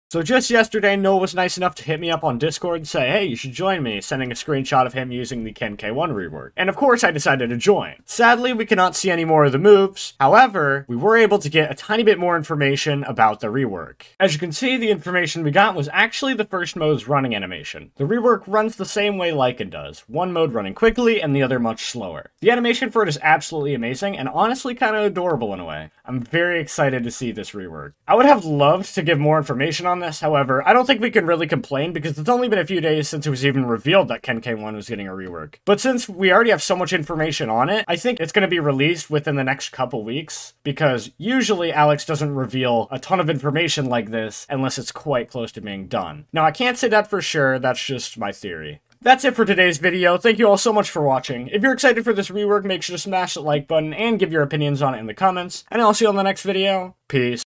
ReKenK1's Run Is INSANE!
In this video I was able to see ReKenK1's new run and It looks awesome!